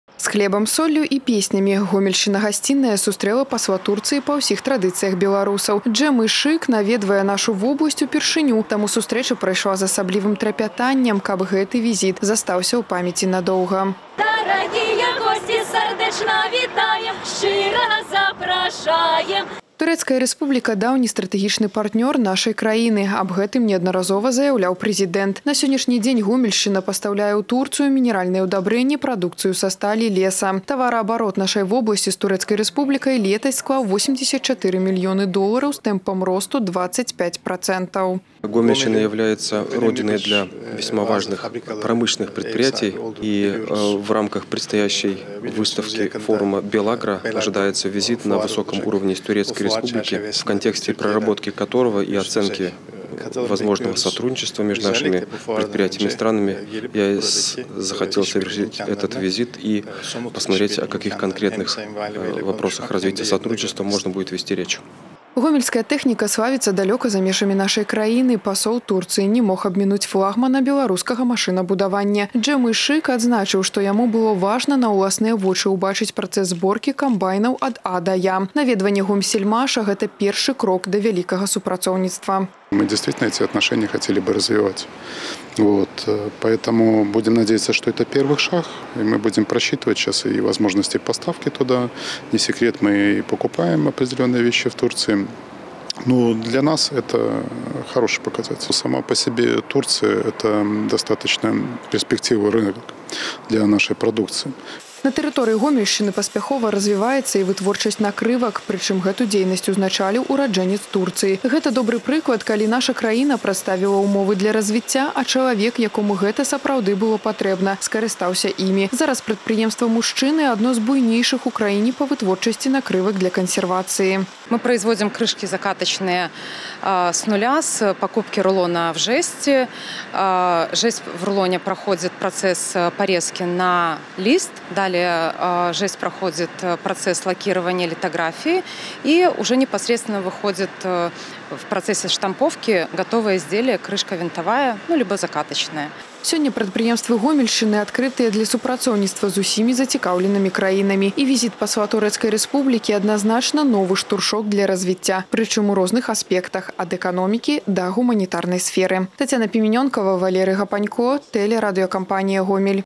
Гомельщину с делегацией посетил посол Турции Гючлю Джем Ышык
Программа визита насыщенная – от продуктивного разговора с главой региона до посещения предприятия по производству крышек для консервации.

С хлебом-солью и песнями – Гомельщина-гостеприимная встретила посла Турции по всем традициям белорусов. Джем Ышик посещает нашу область впервые, поэтому встреча прошла с особым трепетом, чтобы этот визит остался в памяти надолго.